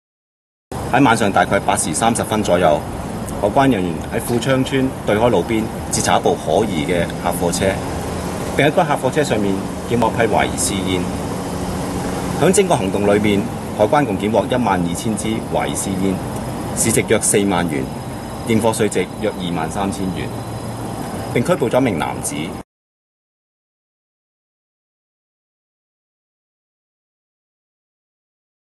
貨Van速遞私煙 海關拉派貨男
香港海關稅收及一般調查科私煙調查組根據線報及深入調查，昨晚(17日)於深水埗富昌邨富旺樓外，截查一輛目標豐田HIACE客貨車，關員在車內搜獲約12,000支未完稅香煙，市值約40,000港元(應課稅值約23,000港元)，20餘歲貨Van司機被捕，檢獲私煙相信供應西九龍區，海關將調查私煙來源。

被捕男子報稱無業，為客貨車登記車主，他被帶返海關總部通宵扣查。